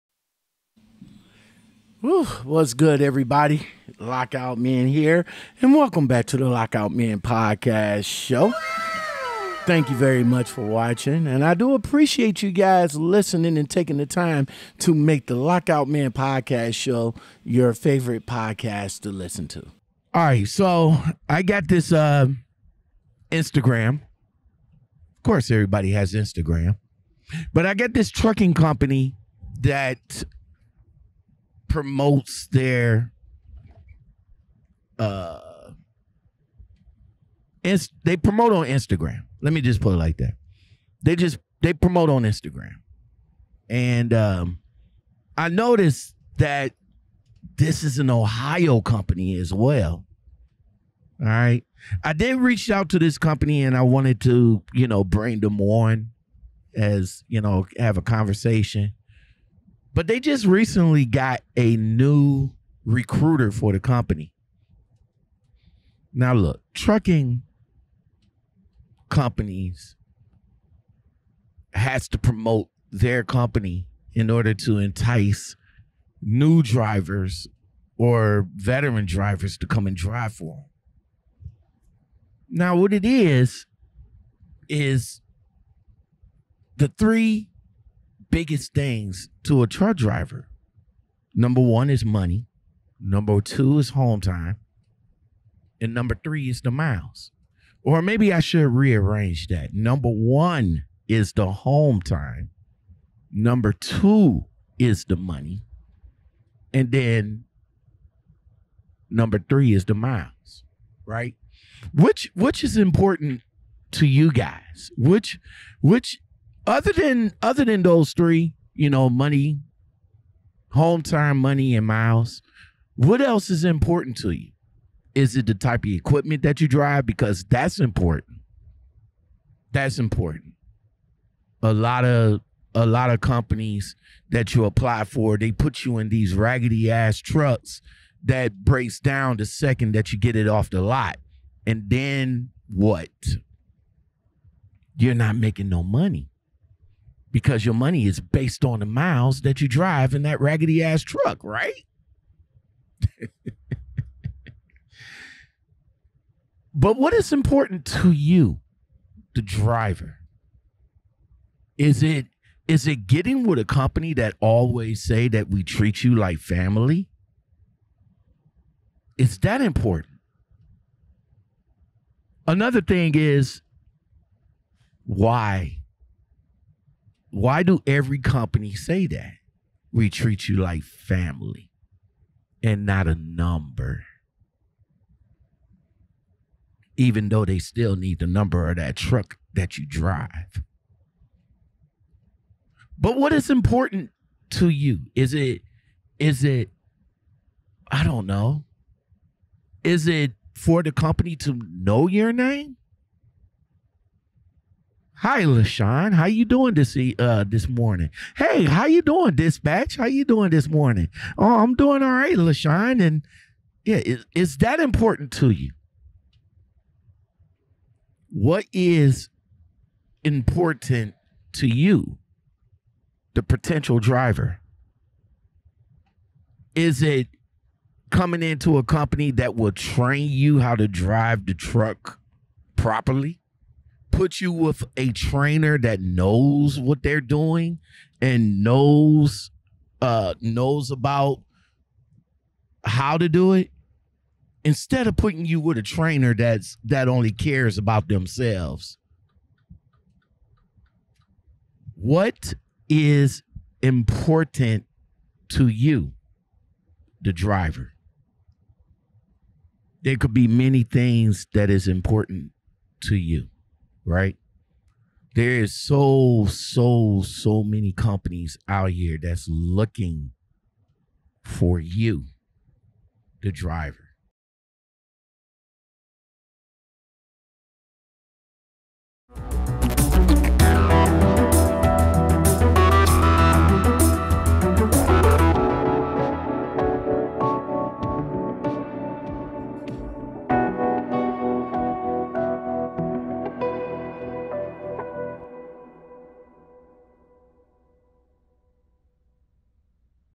What's Important To You
On Todays Lockoutmen Podcast, Lockoutmen  Asks What's Important To You? Money? Miles? Hometime? Truck? What’s What's Important To You??

The best conversation starts here!!!
Join the conversation!!!

Channel Support:
Thanks To Artur Express Trucking For Sponsoring This Channel for more info and to appy here: